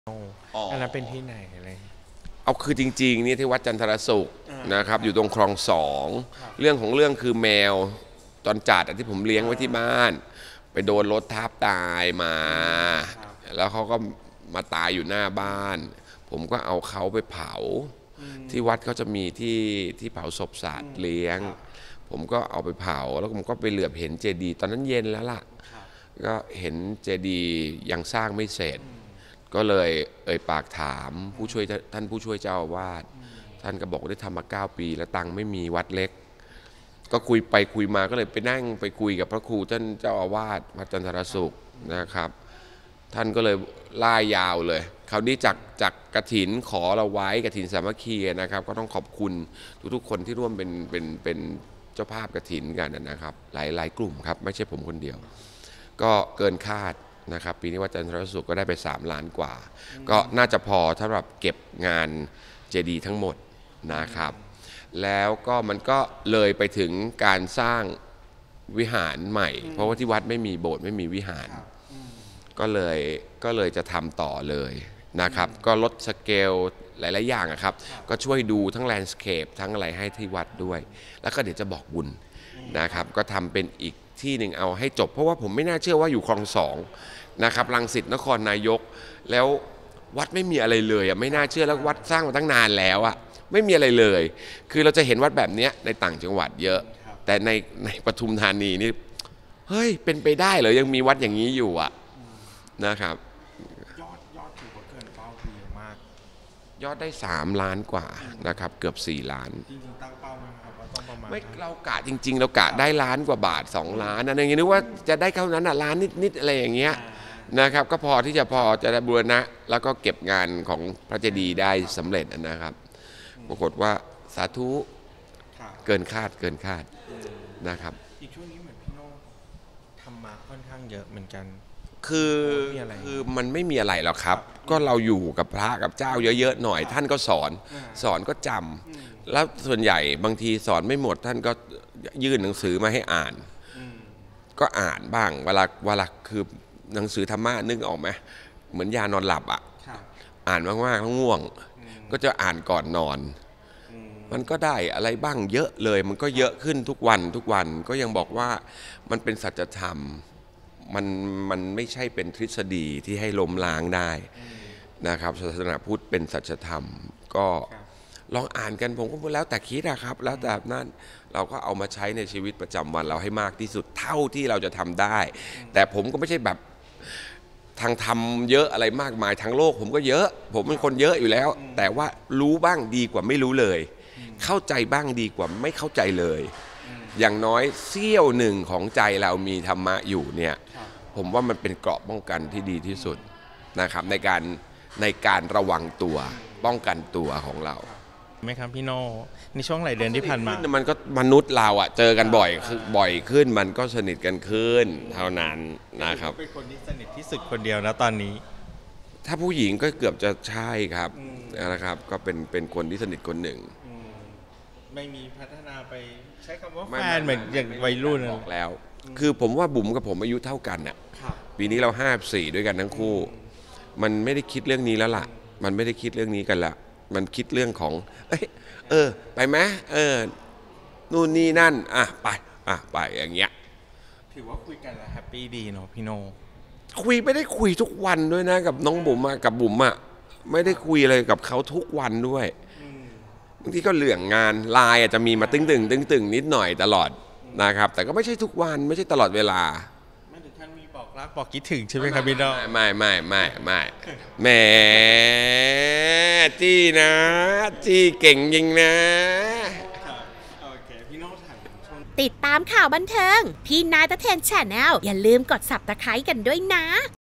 “นีโน่” เผยจุดเริ่มต้นงานทอดกฐิน อัปเดตความสัมพันธ์ “บุ๋ม ตรีรัก” (คลิปจัดเต็ม)
ฟังจากปากนักแสดงรุ่นใหญ่ “นีโน่ เมทนี” ถึงจุดเริ่มต้นในการร่วมทำบุญบูรณะหลังคาเจดีย์วัด พร้อมกับอัปเดตสถานะหัวใจที่แว่วว่า กำลังปลูกต้นรักกับนางแบบรุ่นใหญ่ “บุ๋ม ตรีรัก” จริงหรือเปล่า อยากรู้ต้องฟัง